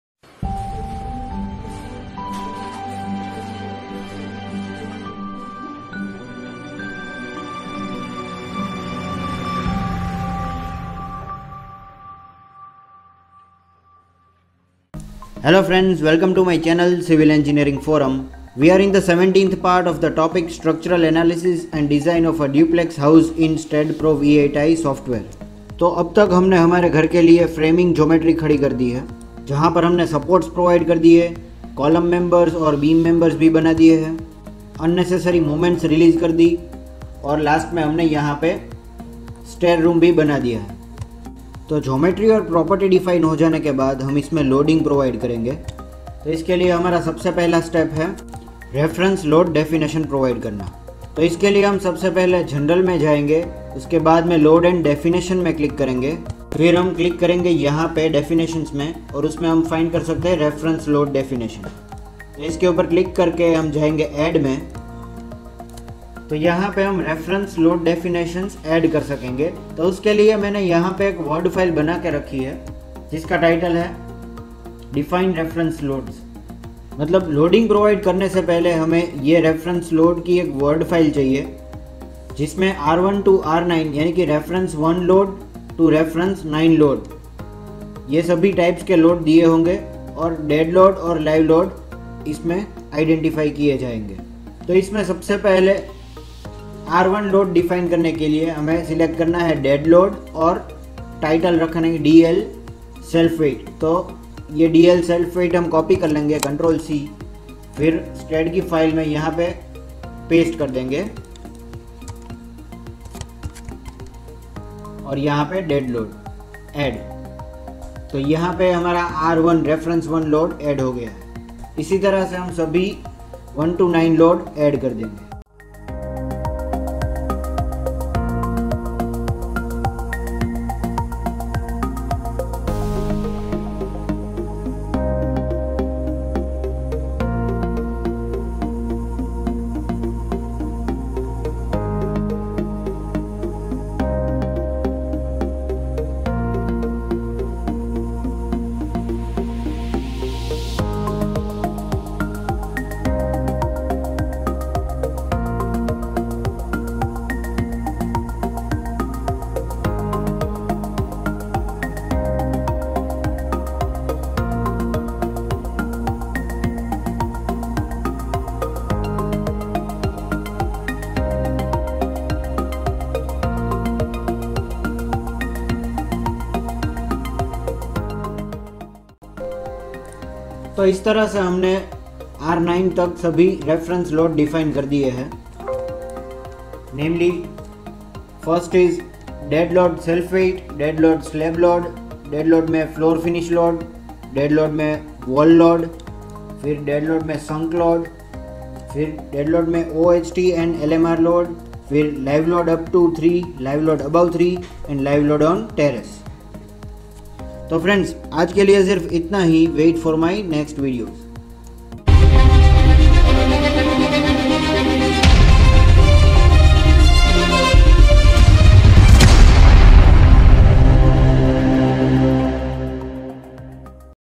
Reference load definition (Structure Design in STAAD Pro V8i - Part 17)
In the part 17 of structure design of a duplex house in STAAD Pro. V8i i have show reference load definition which we need to define before the load combination and after the geometry is been created and property is applied.